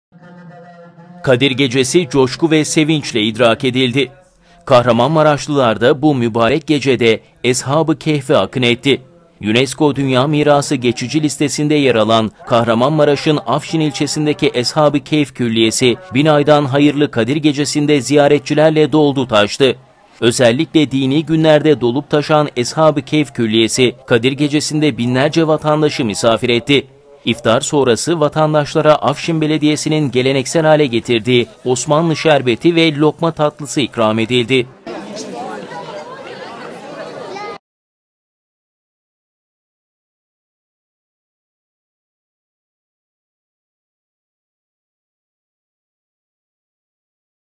KAHRAMANMARAŞLILAR KADİR GECESİ’NDE ESHAB I KEHF’E AKIN ETTİ
Kadir Gecesi, coşku ve sevinçle idrak edildi.
Kahramanmaraşlılar da bu mübarek gece de Eshab-ı Kehf’e akın etti.
UNESCO Dünya Mirası Geçici Listesi’nde yer alan Kahramanmaraş’ın Afşin ilçesindeki Eshab-ı Kehf Külliyesi, bin aydan hayırlı Kadir gecesinde ziyaretçilerle doldu taştı.
Özellikle dini günlerde dolup taşan Eshab-ı Kehf külliyesi, Kadir gecesinde binlerce vatandaşı misafir etti.
İftar sonrası vatandaşlara Afşin Belediyesinin geleneksel hale getirdiği Osmanlı Şerbeti ve lokma tatlısı ikram edildi.